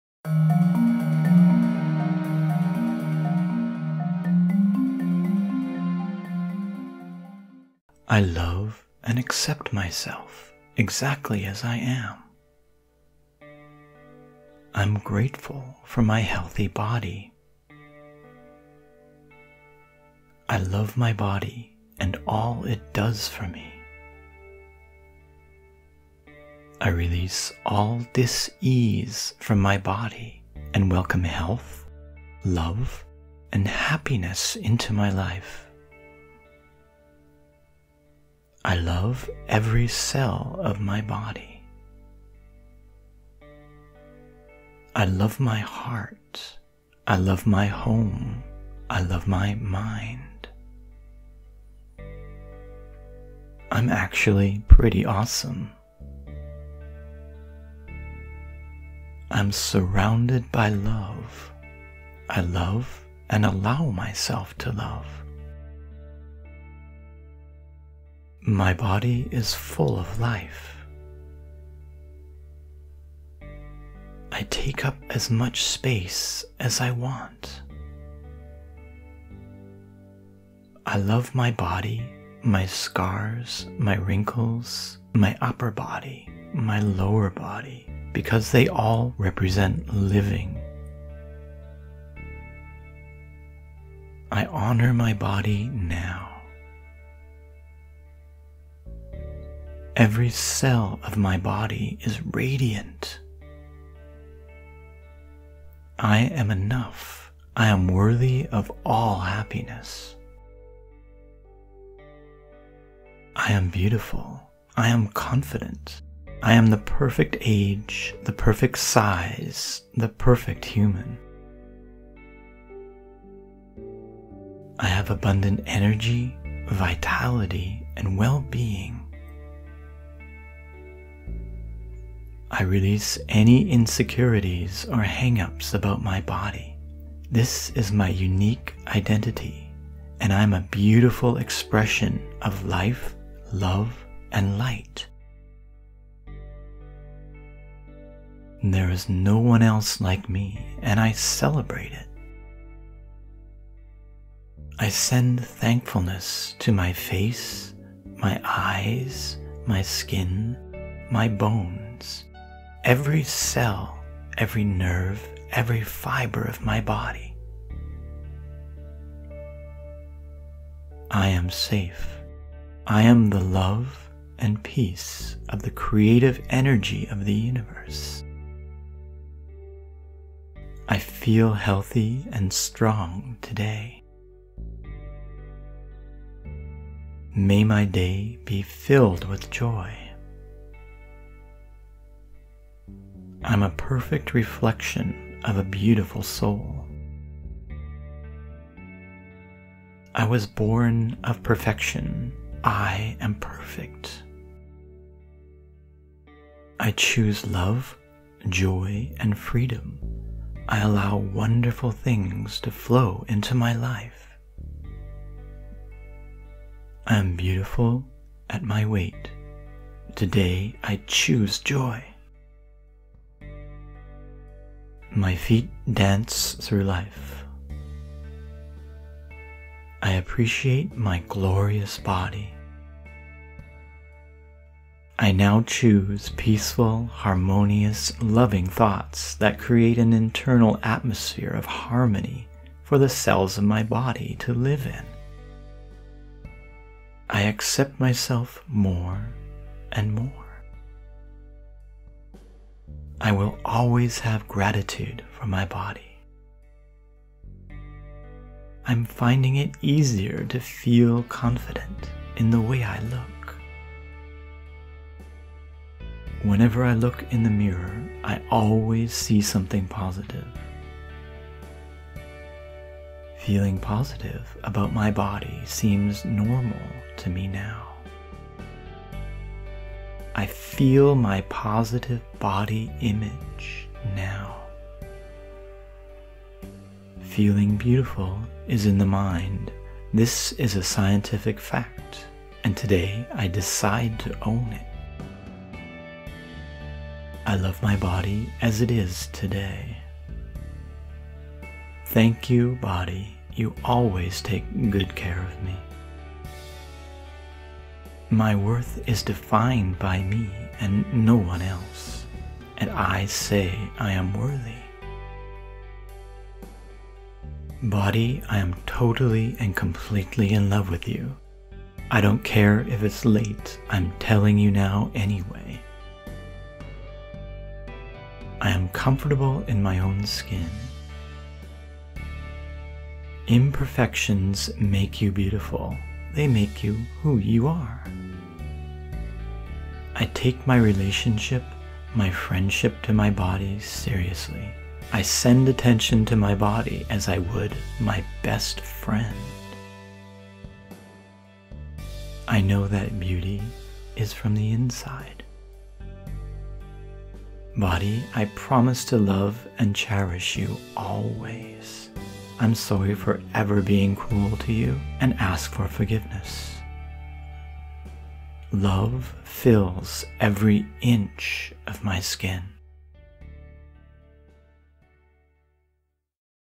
Affirmations: Positive Body Self Image. Turn Body Hate into Body Love
These affirmations help you love yourself today.